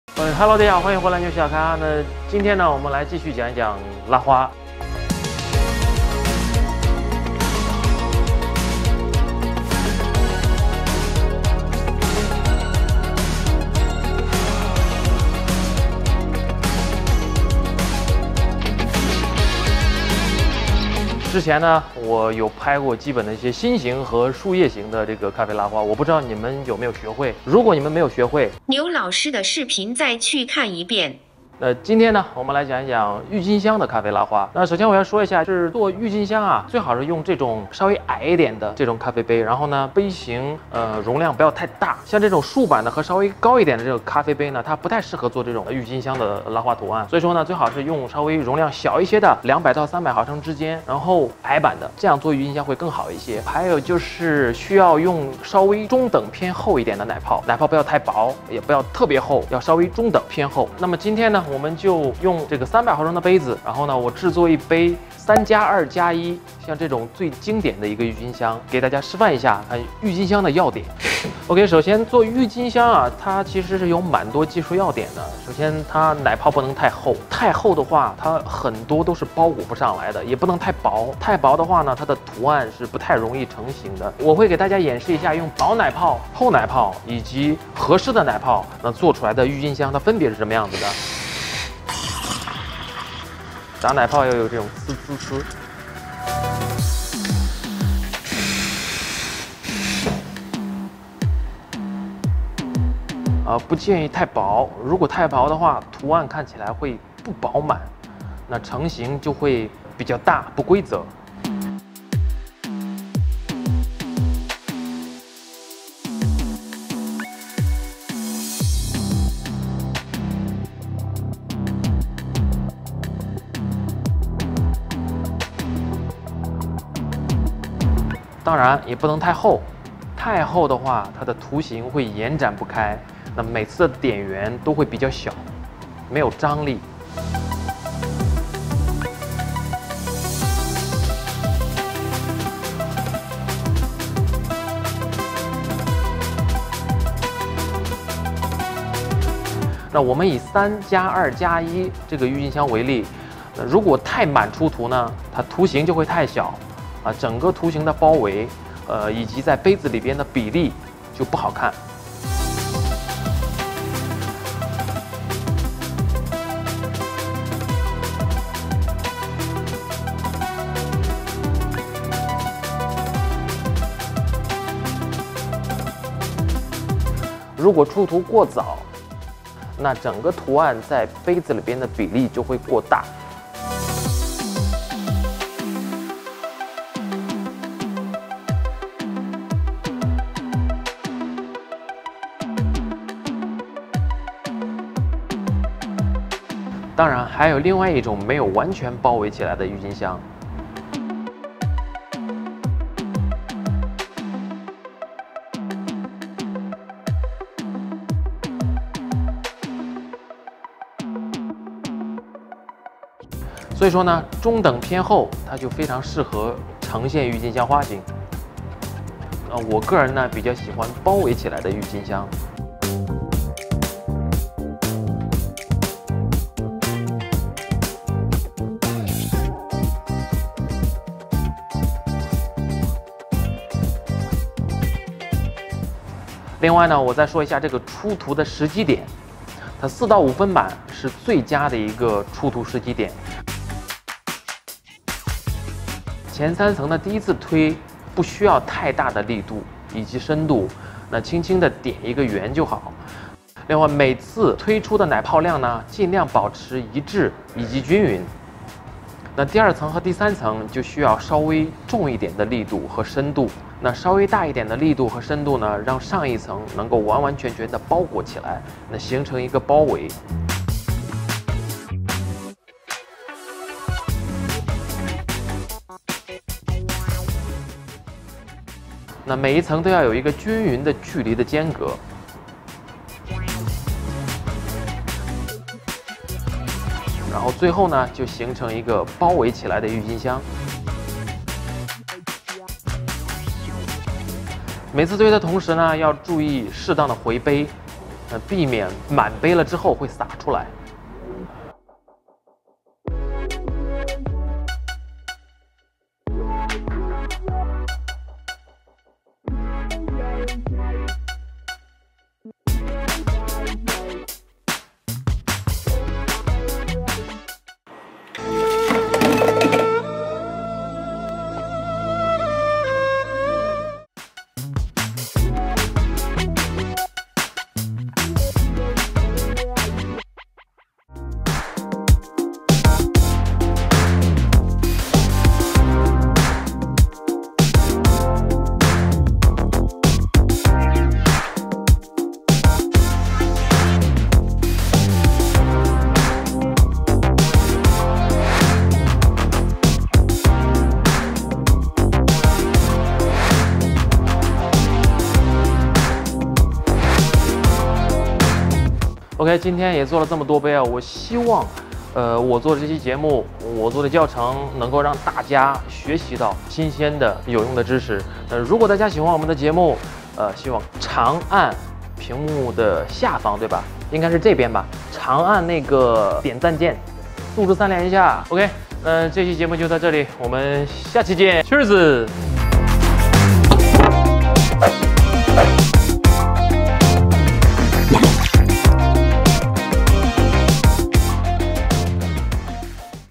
2分钟美味咖啡 | Tulip latte art，全方位演示怎样制作一杯完美的郁金香拉花！
心形和树叶之后的第三个拉花教程来咯！
除了正确版的示范外，还模拟了奶泡过薄过厚，满杯情况不对等状况下的拉（开）花效果，各位小伙伴可以对照着找到自己的问题。
多多练习，天天开花